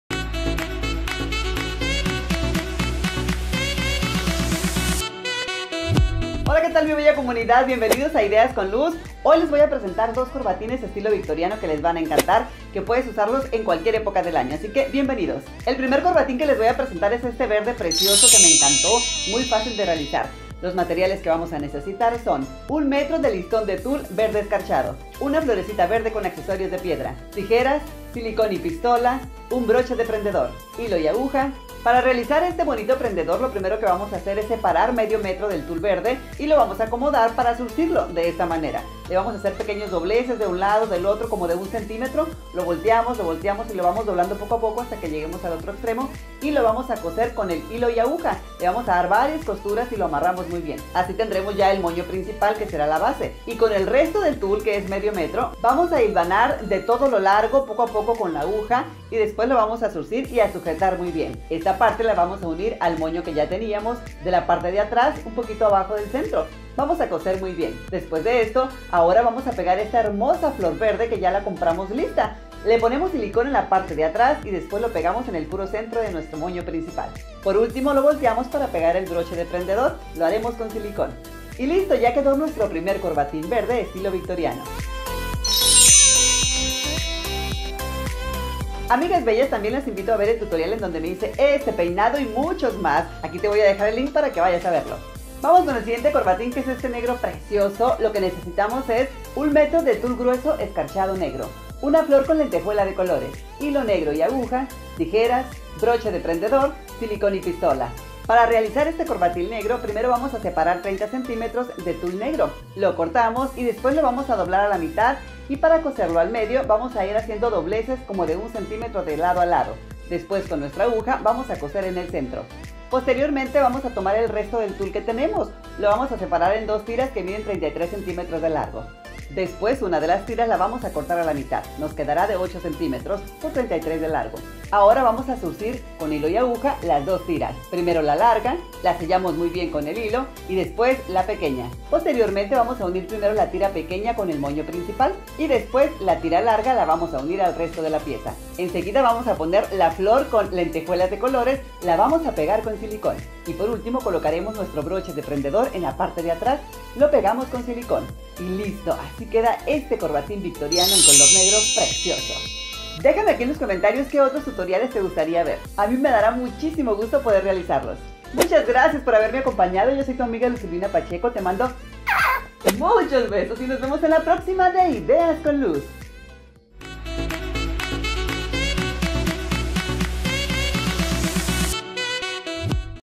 🌸 2 CORBATINES VICTORIANOS PARA DAMA 🌸 2 VICTORIAN TIES FOR LADY 🌸
Hola amigos de Ideas con Luz, hoy les presento 2 corbatines muy hermosos son estilo Victoriano, espero te guste, se ven muy elegantes.